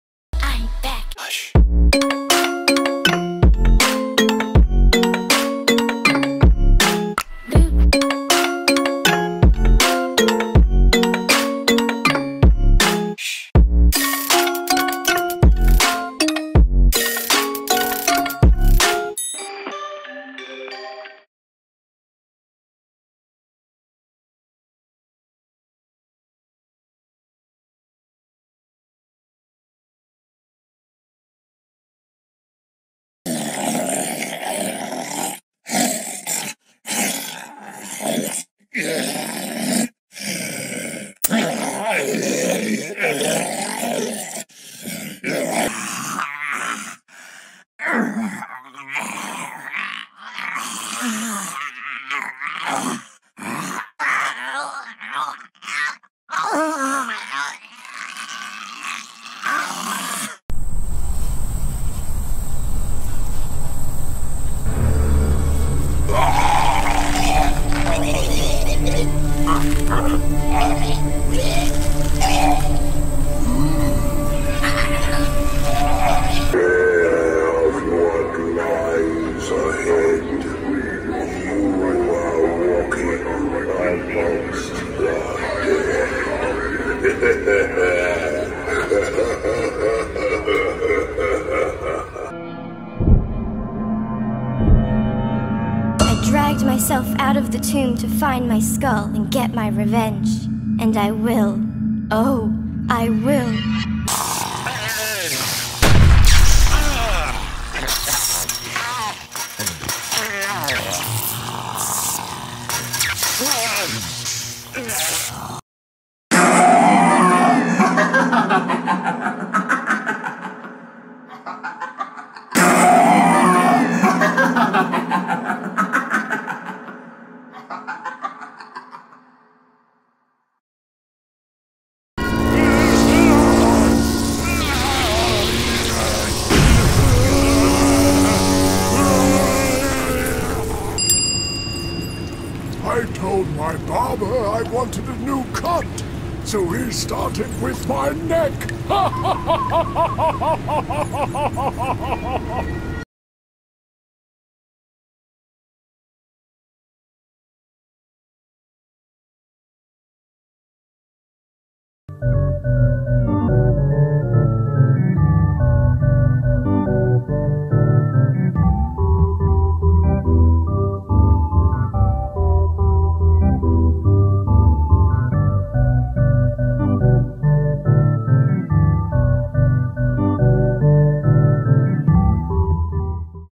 How Spirit Halloween Animatronics Died In Their Backstories.. | Conjure Maven
🎃 Welcome to the video description! 🎃
This is a video about Spirit Halloween's incredible backstory writing for their animatronics. I love that Spirit does this, because it makes the animatronics more like living people, giving them names and backstories that will make you want to buy them even more. It's one of my favorite parts of Spirit Halloween, the 'lore' and the backstories! And I thought it would be interesting if I made a video about how some of them died.

🎃 Sound credits 🎃

Can't find what you're spookin' for? See you at the next conjuring! 🎃